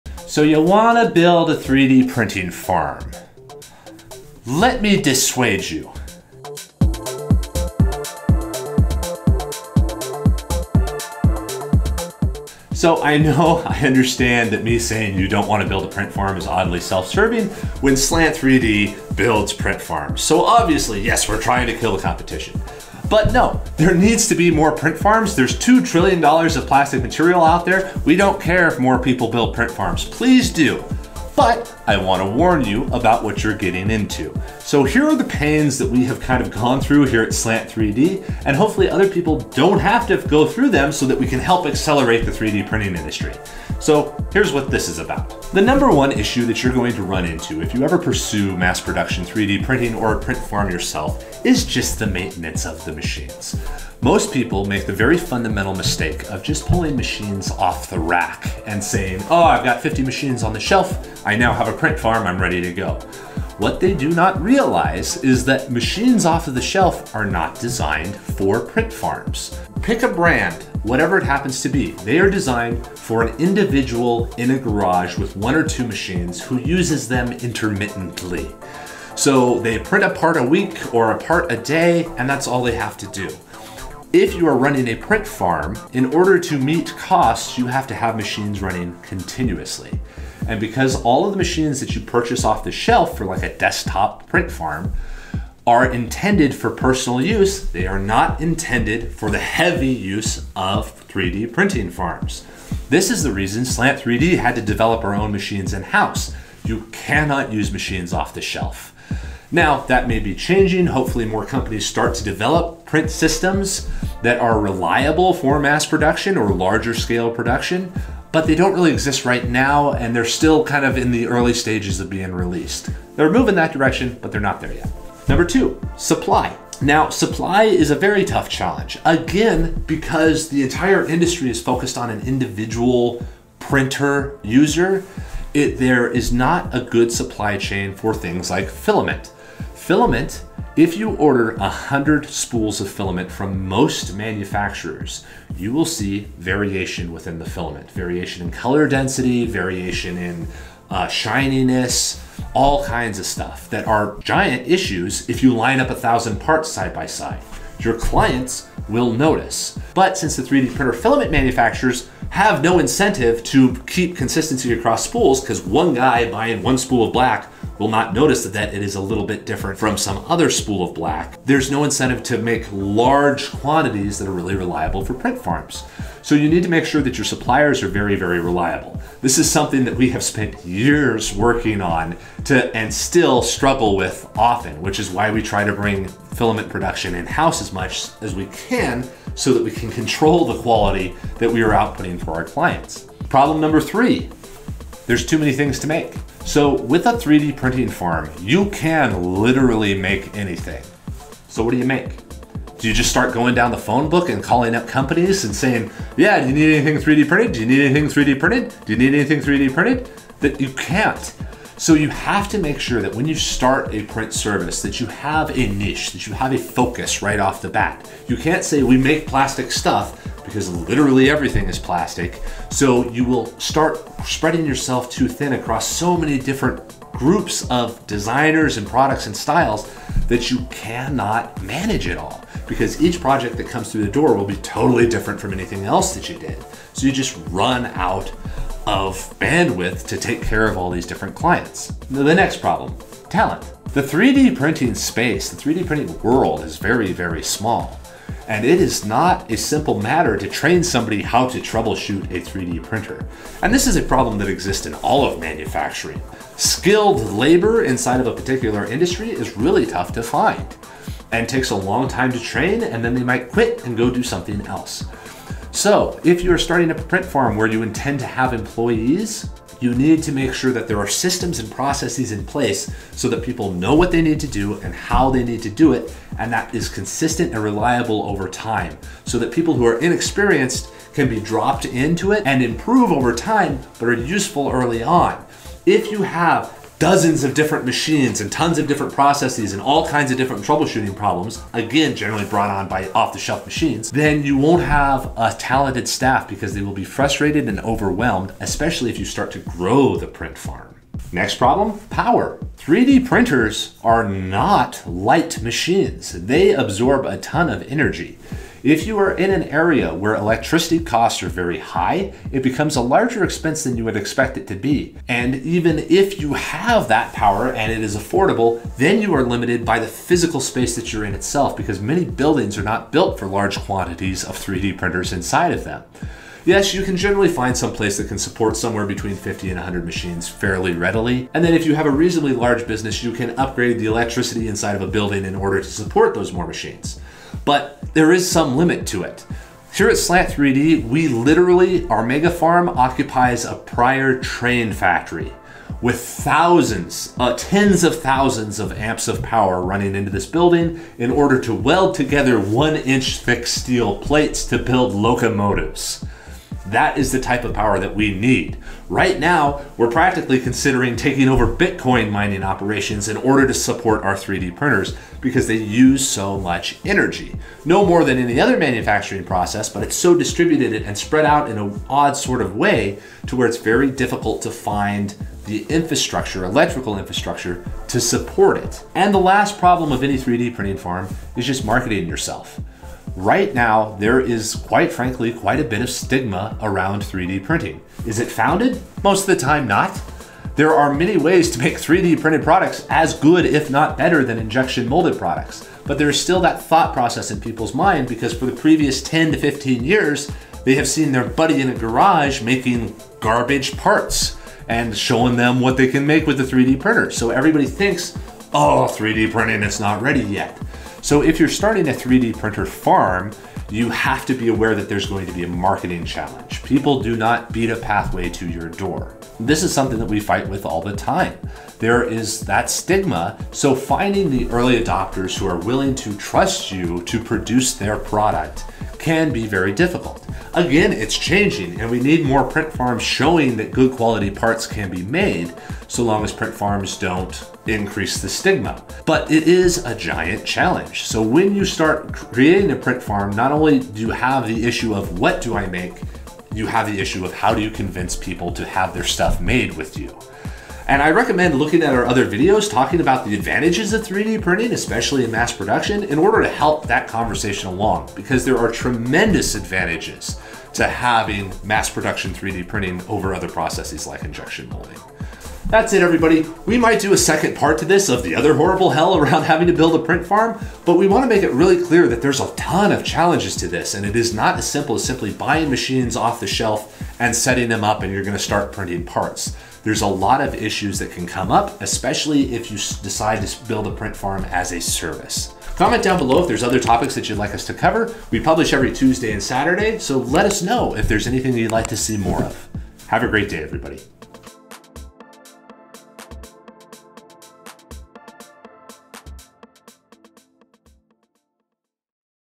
So You Wanna Build a GIANT 3D Print Farm?
In this video, we delve into the trials of building a massive 3D printing farm. From managing machine maintenance and material supply to dealing with high electrical demands and staffing. We explore the challenges and solutions for scaling up a 3D printing operation. Join us as we share insights, strategies, and best practices for tackling the hurdles of large-scale 3D printing production. Whether you're an entrepreneur, engineer, or maker, this video will help set the expectation for the challenges you will face while running a giant 3D printer farm. Watch now to learn more!